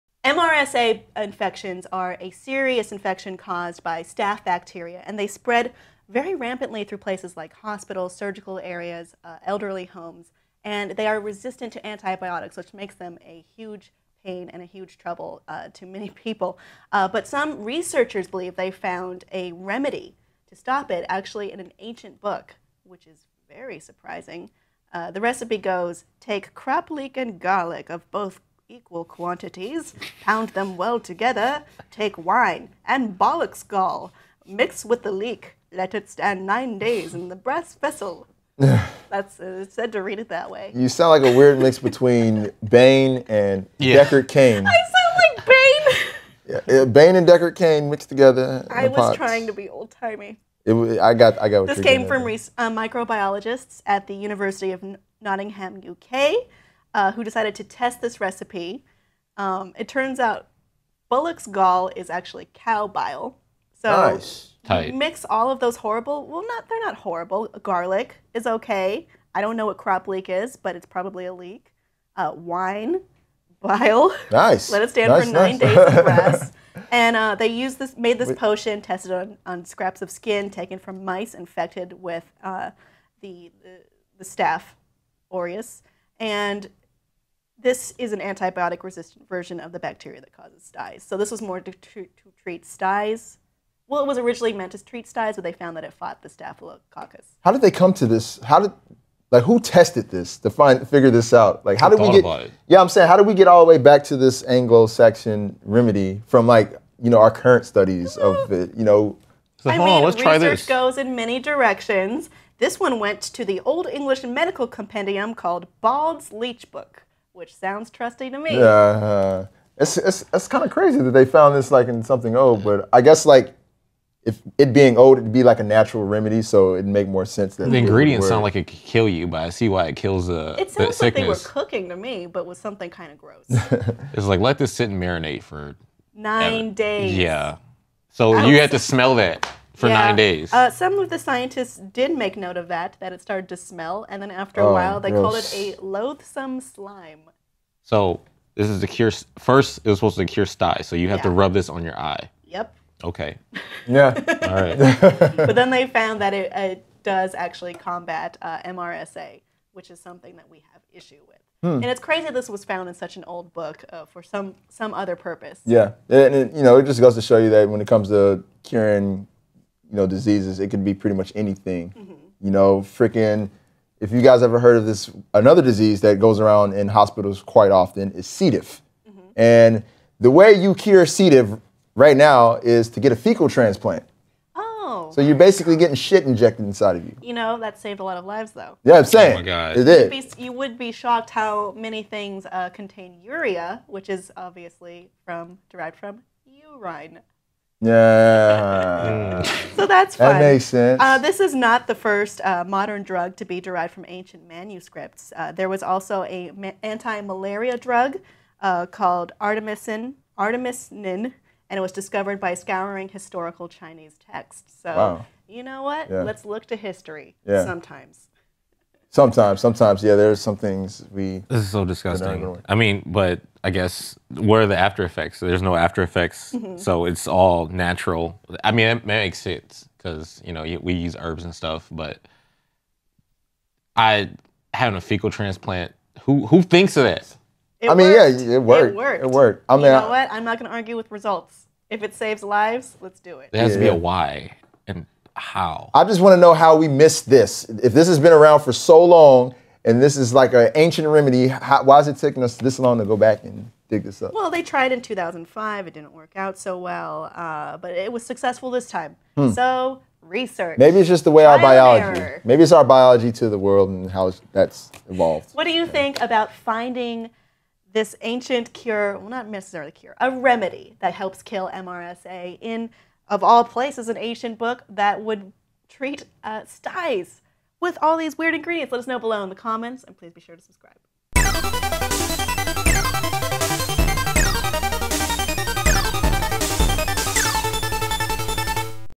Weird 1,000 Year-Old Recipe Kills MRSA (Staph) Superbug
The Anglo-Saxons were well known for a lot of historical feats, but curing antibiotic resistant superbugs responsible for deadly Methicillin-resistant Staphylococcus aureus infections is definitely a new one. Which strange remedy did the trick?

Kim Horcher discusses with special guests Malik Forte (Gaming Editor, Nerdist.com) and Carl White (Pro-gaming world champion "Perfect Legend" )